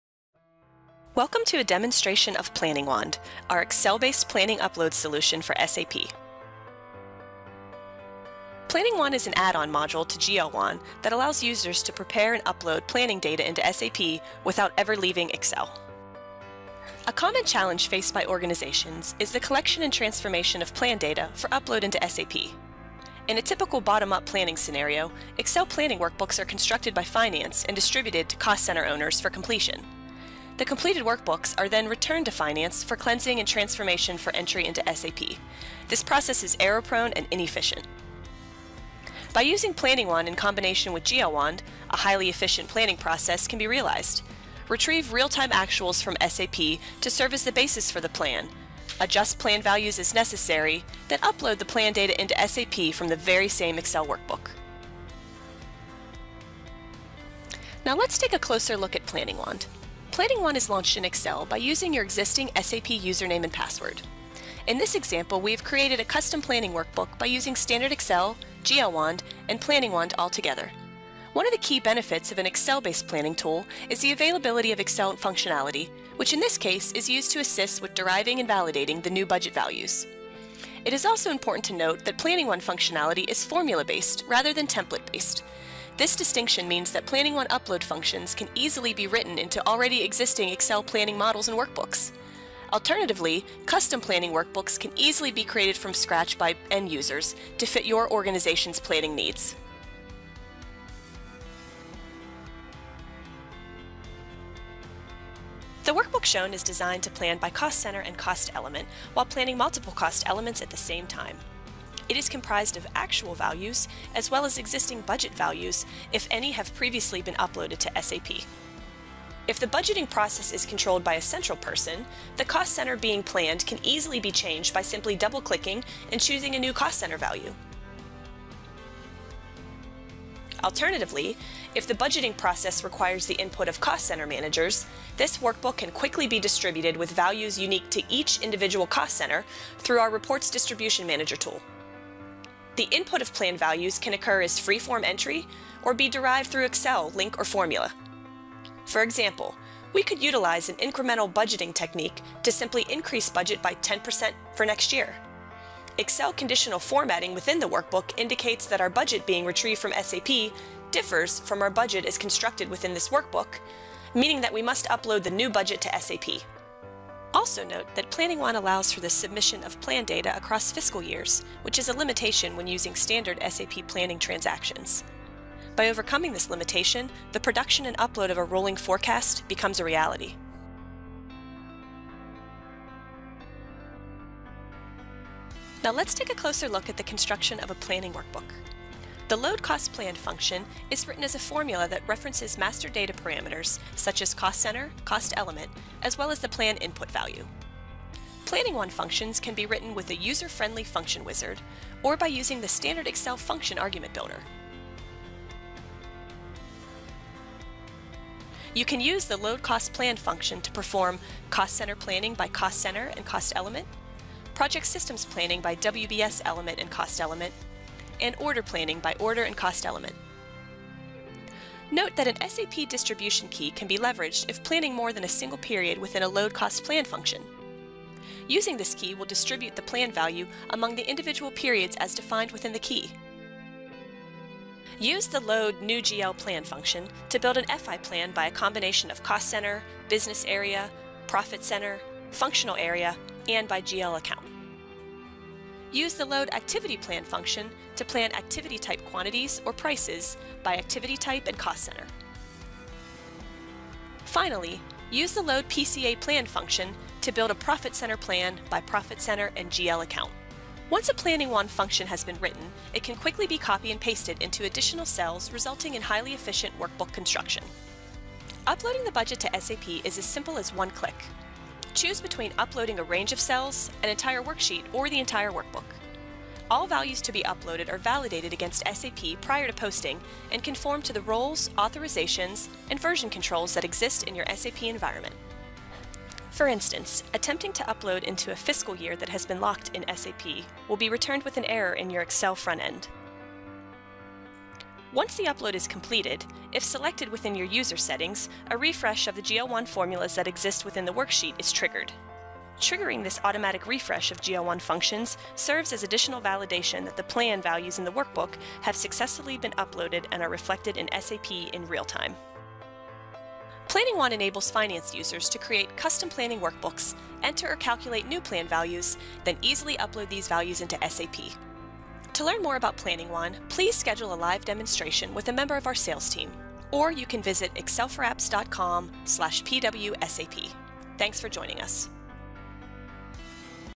Planning Wand for SAP Video
Planning Wand is an add-on module to GL Wand which allows SAP customers to easily upload budget planning data from Excel into SAP. Planning Wand and GL Wand eliminate manual data extracts and uploads to enable faster planning and forecasting processes in Excel. Pre-integrated with SAP, Planning Wand includes an easy Load Cost Plan Wizard for creation of an Excel function, and real-time validation and error correction before uploading to SAP.